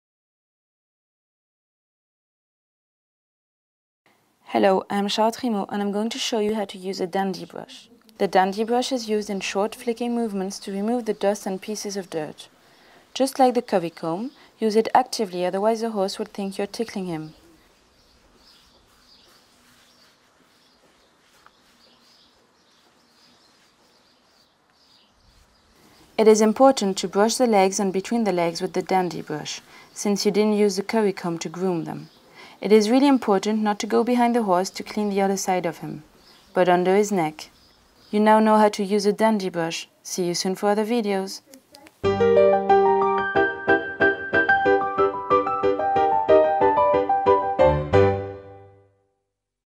Horse Riding - How to use the dandy brush
The dandy brush is used in short, flicking movements to remove the dust and pieces of dirt. Just like the curry comb, use it actively otherwise the horse will think you are tickling him. It is important to brush the legs and between the legs with the dandy brush, since you didn't use the curry comb to groom them. It is really important not to go behind the horse to clean the other side of the horse, but under his neck. You now know how to use a dandy brush!